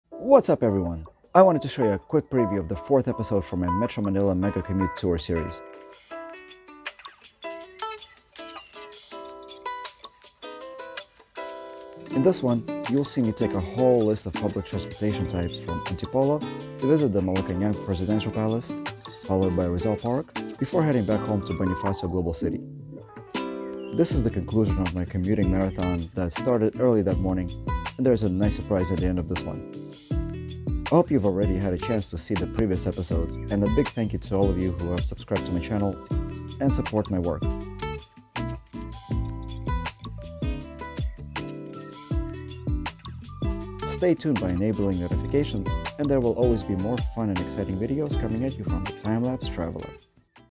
Metro Manila Mega Commute | Episode 4 Preview
This is a preview of the 4th episode of my Metro Manila Mega Commute Tour where I left from Antipolo City to visit the Malacanan Presidential Palace and the Rizal park and memorial monument before heading back home to Bonifacio Global City. Along the way, I commuted by a buses,  trains and tricycles, besides all the walking around through city streets of several different neighborhoods.
Although I haven't actually gotten close enough to the Malacañang Palace, I did have an amazing experience of touring the area around it, at least to soak in the environment and get to know the local city streets a little better.
Rizal Park on the other hand, was booming with excitement for the commemoration of People Power Revolution, which ousted the late dictator Ferdinand Marcos, only to prepare the highest office for his son, Ferdinand Marcos Jr.